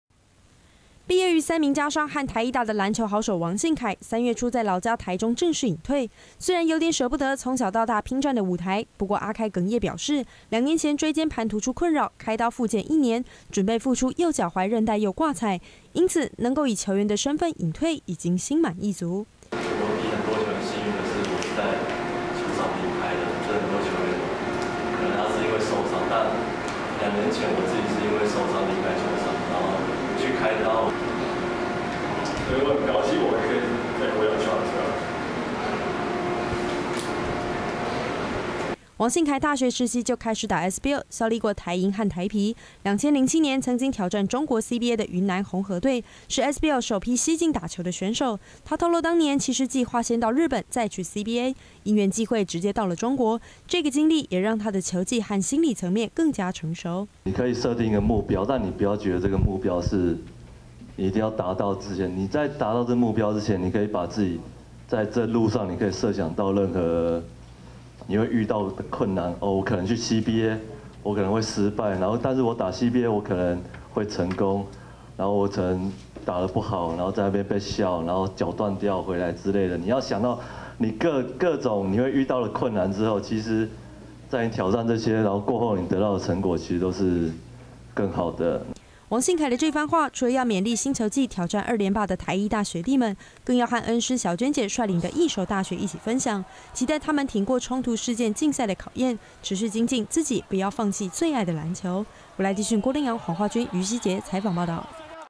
3/23 王信凱正式引退 結束選手生涯
籃球好手王信凱3月初正式引退，這位前台啤球星過去打過SBL也曾經到中國CBA打拚，兩年前因為腰傷淡出球場，他以自己的例子勉勵所有籃球路上的小學弟，雖然最終成就不算特別出色，但追尋目標的過程將獲得許多收穫，因此千萬不要輕易放棄自己的理想